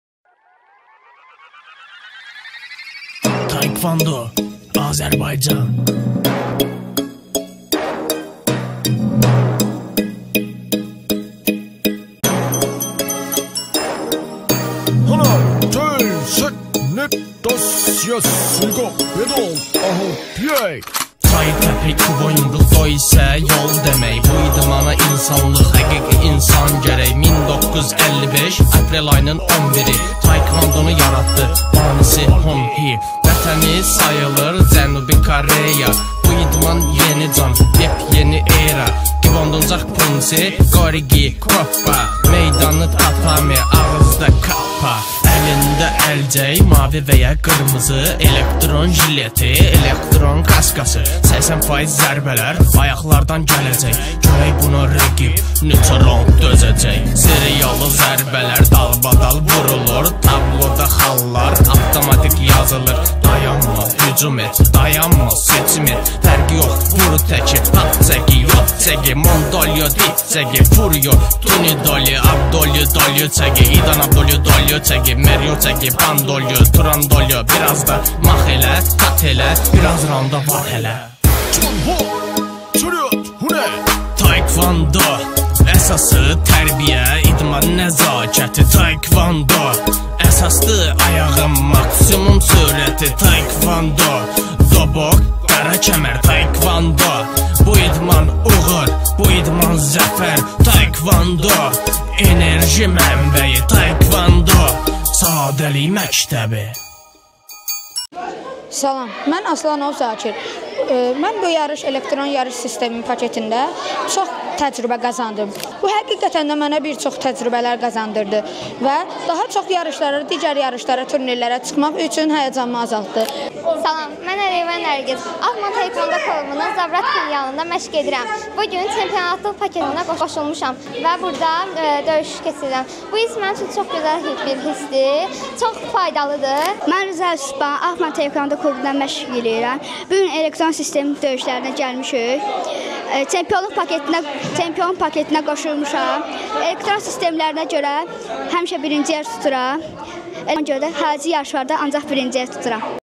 Elektron sistemlə döyüş günü
Qonaq komanda: Biləsuvar🤝 @bilesuvar_taekvondo_mektebi

“Çempion zirvəsi” və “Elit atlet” məşq paketlərinin istifadəçilərinin rəyi✅